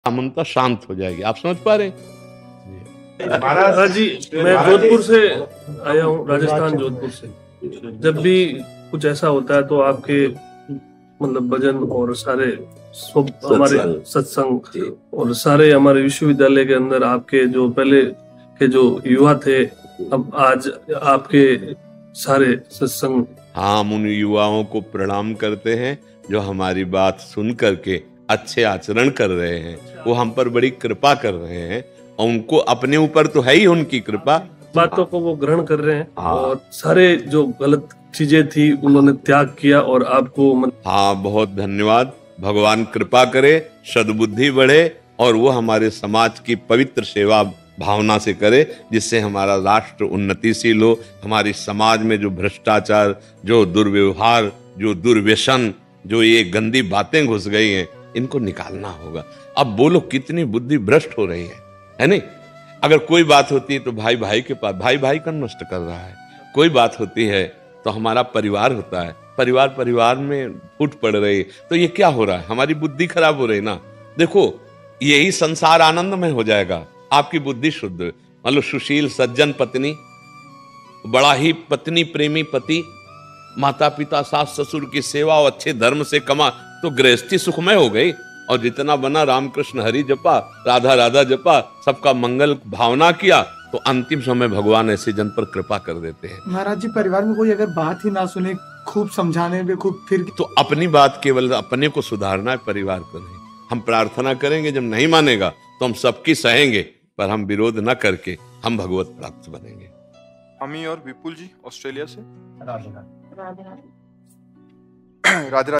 प्रश्न-592 : महाराज जी मैं राजस्थान विश्वविद्यालय से आया हूं हम अपने बच्चों को motivate व...🙏🙏
प्रश्न-592 : महाराज जी मैं राजस्थान विश्वविद्यालय से आया हूं हम अपने बच्चों को motivate व गलत आचरण से बचने के लिए आपकी videos दिखाते है।🙏🙏

🌹🌷जय श्री राधावल्लभ जय श्री हित हरिवंश ।🌷🌹
🌼🌻जय जय श्री वृन्दावन धाम ।।🌻🌼